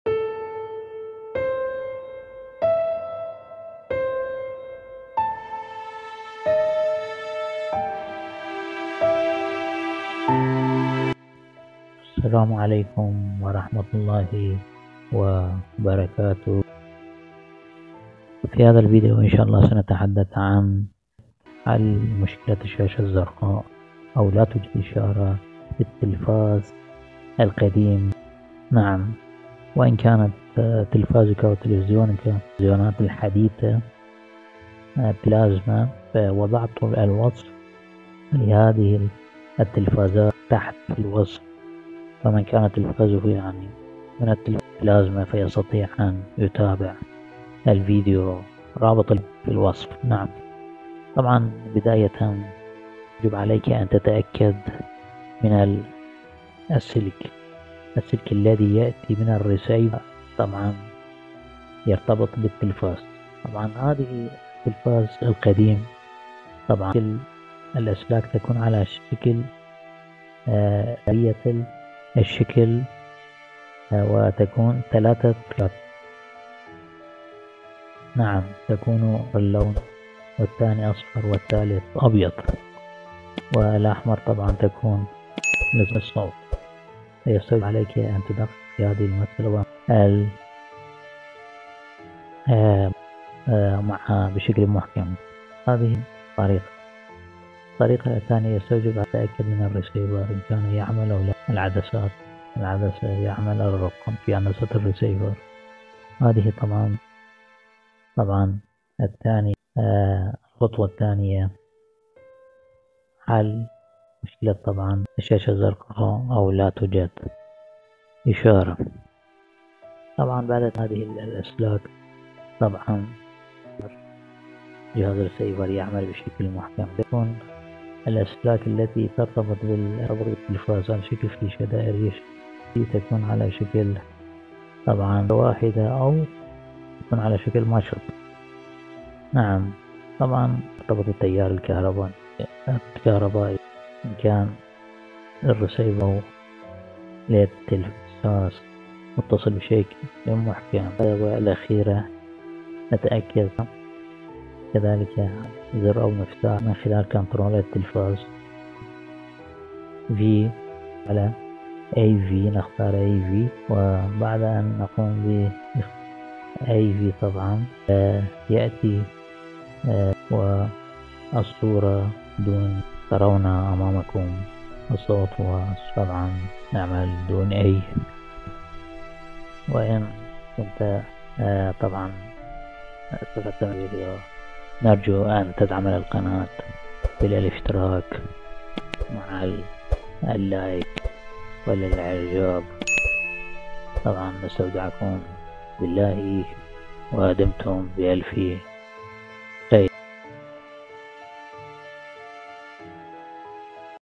حل مشكلة الشاشة الزرقاء blue screen | حل مشكلة لا توجد إشارة no signal في التلفزيون القديم(2025).
هذا رابط فيديو "حل مشكلة لا توجد إشارة no signal | الشاشة الزرقاء" للتلفازات الحديثة (بلازما) 👇: